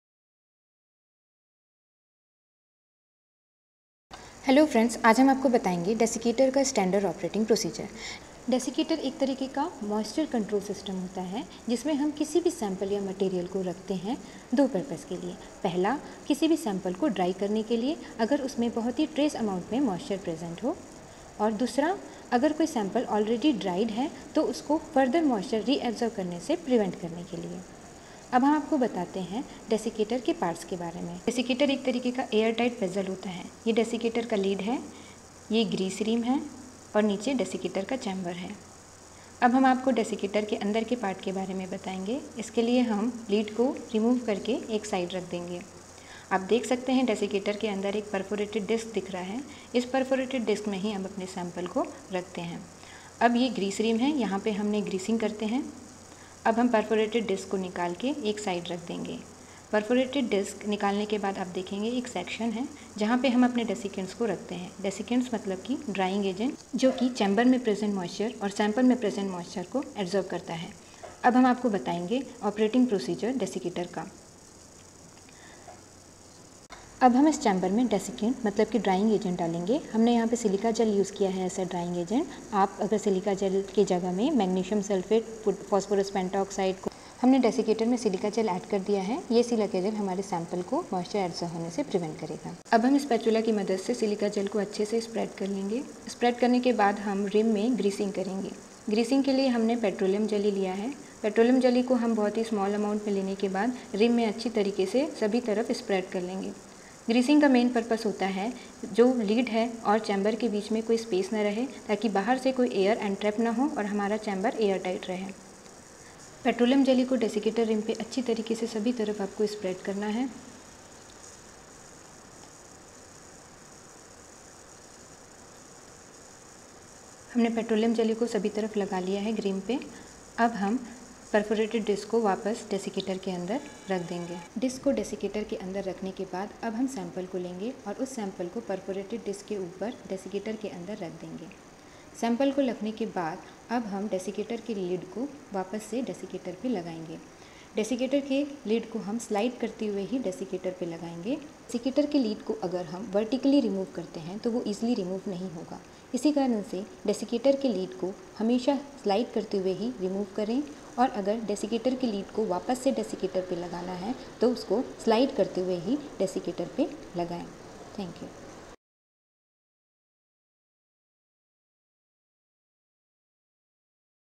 Desiccator = Principle, Working and Standard Operating Procedure | How to Use Desiccator Properly
Desiccator - Principle, Working and Standard Operating Procedure (ENGLISH) By Solution Pharmacy

In pharmacy or any other science experiments, we need to preserve the drug or active constituents from herbal sources we have to keep these in a closed and tight container or vessels. Desiccators is one of those instrument or apparatus which are sealable enclosures containing desiccants used for preserving moisture-sensitive items such as cobalt chloride paper for another use. A common use for desiccators is to protect chemicals which are hygroscopic or which react with water from humidity.

The contents of desiccators are exposed to atmospheric moisture whenever the desiccators are opened. It also requires some time to achieve a low humidity. Hence they are not appropriate for storing chemicals which react quickly or violently with atmospheric moisture such as the alkali metals; a glovebox or Schlenk-type apparatus may be more suitable for these purposes.
Desiccators are sometimes used to remove traces of water from an almost-dry sample. Where a desiccator alone is unsatisfactory, the sample may be dried at elevated temperature using Abderhalden's drying pistol.

Get in touch with solution by just clicking following links-